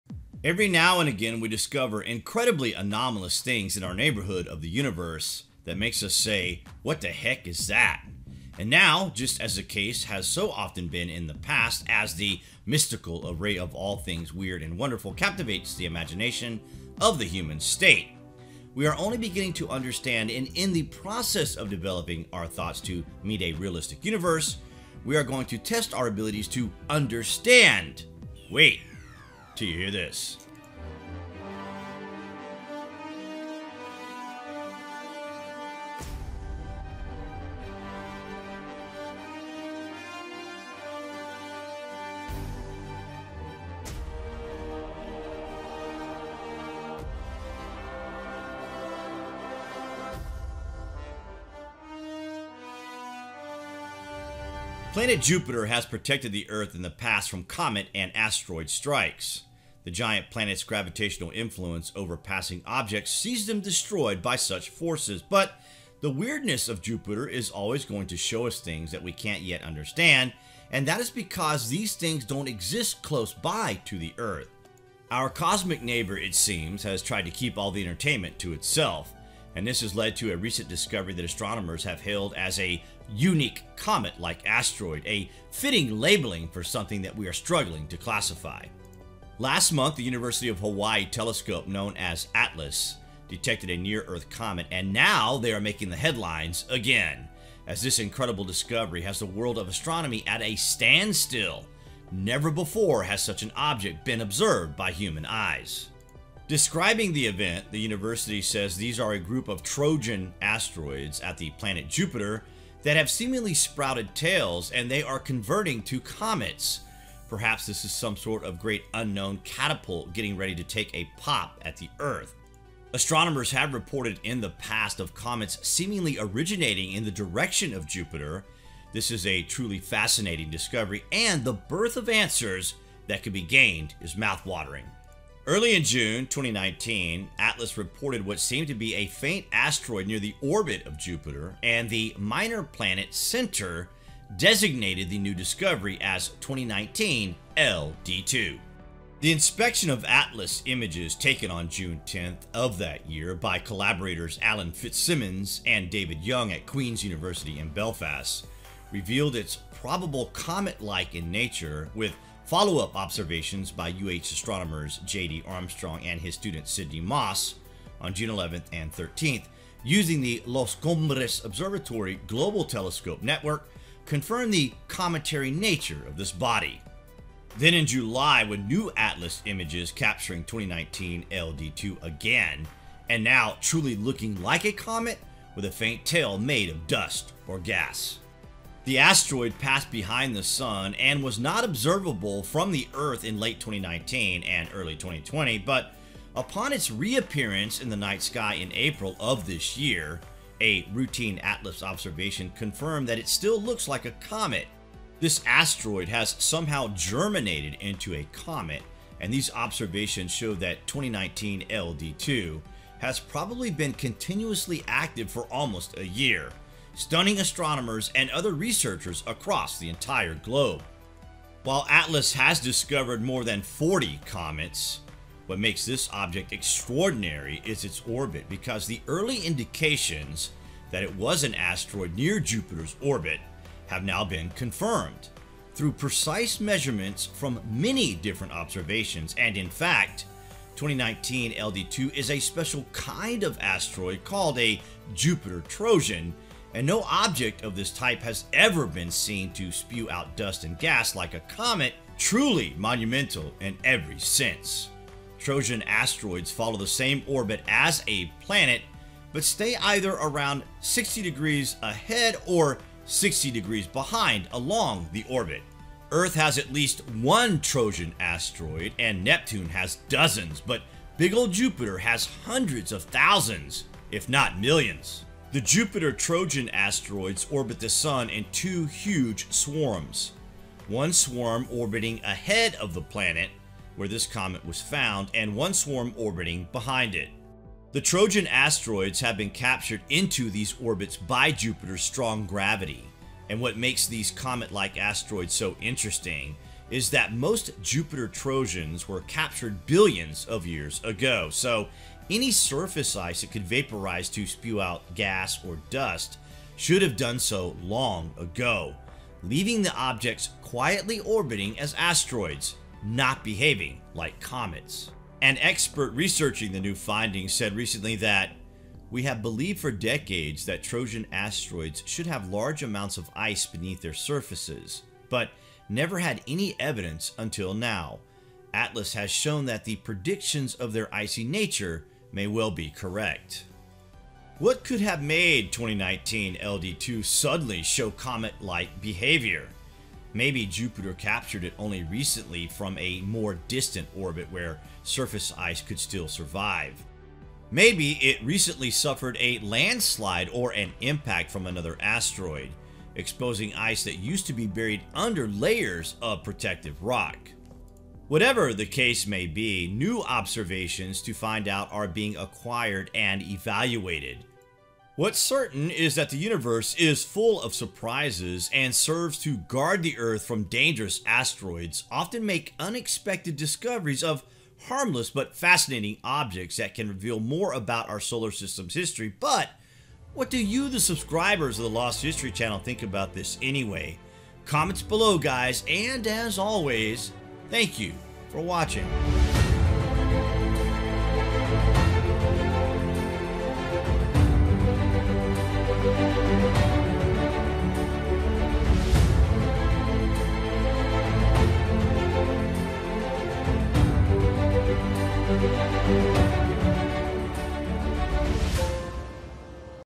Trojan Asteroid at Jupiter Sprouts a 'Comet-Like Tail' (Solar Object P/2019 LD2) Stuns Astronomers!!
Perhaps there is a simple explanation like the Asteroid collided with another object in the Trojan swarm but then again, maybe this is by design, but what do you guys think about this latest discovery?